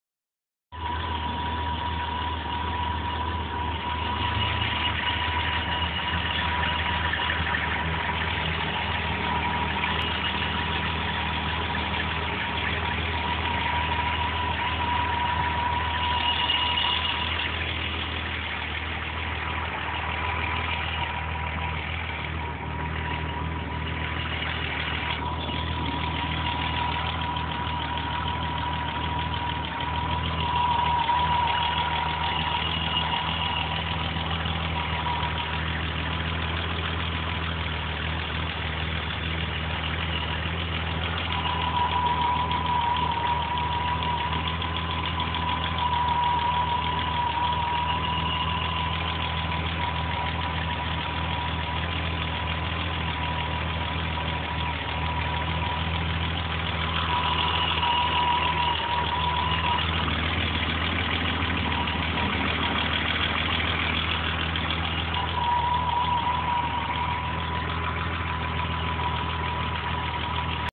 E30 90 325is Project
Running rough despite new spark plugs.  One of the wires had corrosion in it and I tried cleaning it.  It may need a replacement.